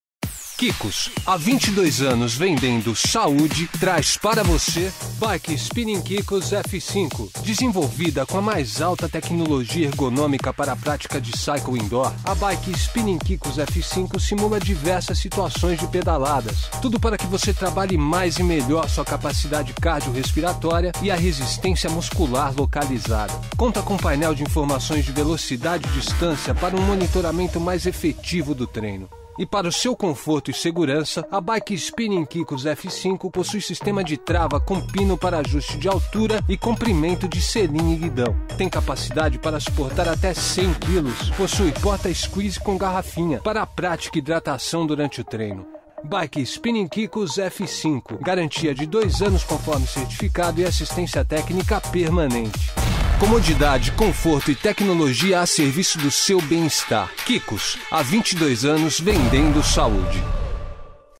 Bicicleta Spinning Kikos F5 com Ajuste de Altura
A Bicicleta Spinning Kikos F5 é para você que quer pedalar sem sair de casa, com simulação de diversas pedaladas. Essa Bike Spinning suporta até 100Kg e possui painel com exibição de velocidade e distância. Confira já incrível oferta

Você que deseja trabalhar resistência muscular localizada através das pedalas, a Bicicleta Spinning Kikos F5 permite ajuste de altura do banco e comprimento em relação ao guidão. E para você se hidratar sem parar os treinos, essa Bike Spinning possui também porta squeeze. Esse equipamento Kikos está à venda no site e aplicativo Centauro para Android e iOS com preço até 12x sem juros, confira